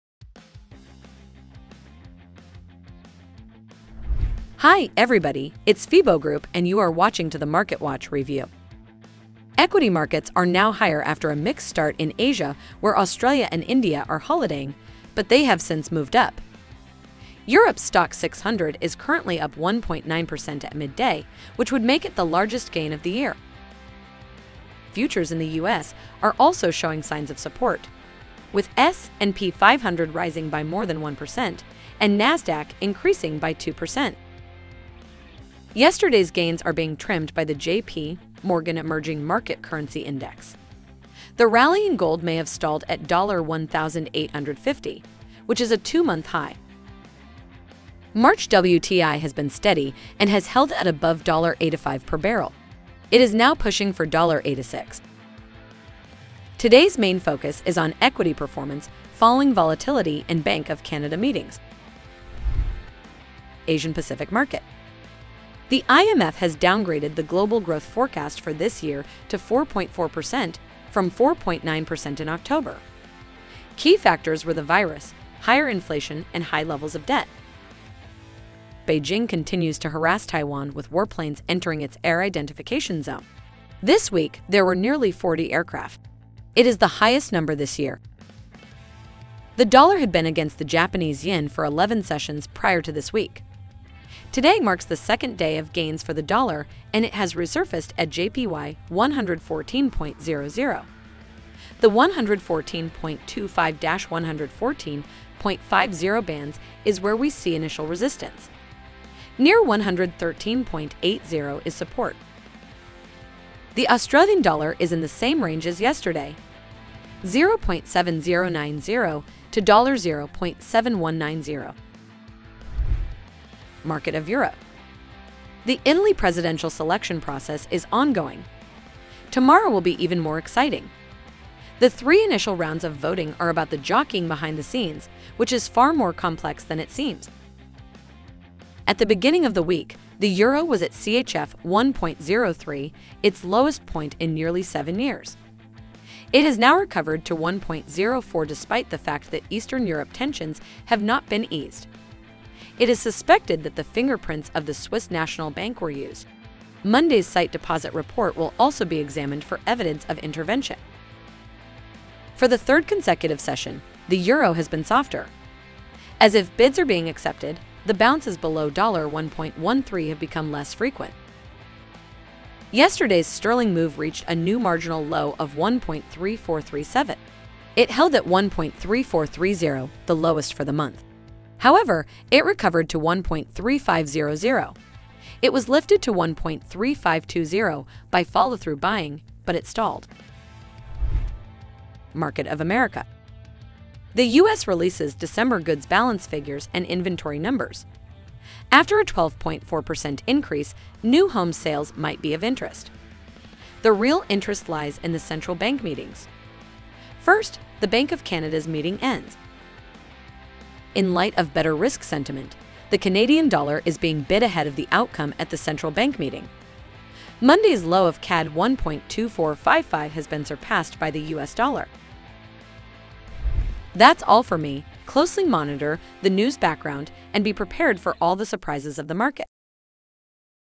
Forex daily analysis from FIBO Group (26/01/2022)
Become a trader with FIBO Group!  Start trading without risk

00:00 | Market Watch by FIBO Group
00:09 | Overview
01:03 | Asia Pacific
02:03 | Europe
03:12 | America

Equity markets are now higher after a mixed start in Asia where Australia and India are holidaying, but they have since moved up. Europe's Stoxx 600 is currently up 1.9% at midday, which would make it the largest gain of the year.
Futures in the US are also showing signs of support, with S&P 500 rising by more than 1%, and NASDAQ increasing by 2%.
Yesterday's gains are being trimmed by the JP Morgan Emerging Market Currency Index. The rally in gold may have stalled at $1850, which is a two-month high.
March WTI has been steady and has held at above $85 per barrel. It is now pushing for $86.
Today's main focus is on equity performance following volatility and Bank of Canada meetings.

Asia Pacific
The IMF has downgraded the global growth forecast for this year to 4.4%, from 4.9% in October. Key factors were the virus, higher inflation and high levels of debt.

Beijing continues to harass Taiwan with warplanes entering its Air Identification Zone. This week, there were nearly 40 aircraft. It is the highest number this year.

The dollar had been against the Japanese yen for 11 sessions prior to this week. Today marks the second day of gains for the dollar and it has resurfaced at JPY114.00. The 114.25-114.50 bands is where we see initial resistance. Near 113.80 is support.

The Australian dollar is in the same range as yesterday: 0.7090 to $0.7190.

Europe
The Italy presidential selection process is ongoing. Tomorrow will be even more exciting. The three initial rounds of voting are about the jockeying behind the scenes, which is far more complex than it seems.

At the beginning of the week, the euro was at CHF1.03, its lowest point in nearly seven years. It has now recovered to 1.04 despite the fact that Eastern Europe tensions have not been eased. It is suspected that the fingerprints of the Swiss National Bank were used. Monday's sight deposit report will also be examined for evidence of intervention.

For the third consecutive session, the euro has been softer. As if bids are being accepted, the bounces below $1.13 have become less frequent.

Yesterday's Sterling move reached a new marginal low of 1.3437. It held at 1.3430, the lowest for the month. However, it recovered to 1.3500. It was lifted to 1.3520 by follow-through buying, but it stalled.

America.
The US releases December goods balance figures and inventory numbers. After a 12.4% increase, new homes sales might be of interest. The real interest lies in the central bank meetings. First, the Bank of Canada's meeting ends.

In light of better risk sentiment, the Canadian dollar is being bid ahead of the outcome at the central bank meeting. Monday's low of CAD1.2455 has been surpassed by the US dollar.

That’s all for me, closely monitor the news background and be prepared for all the surprises of the market.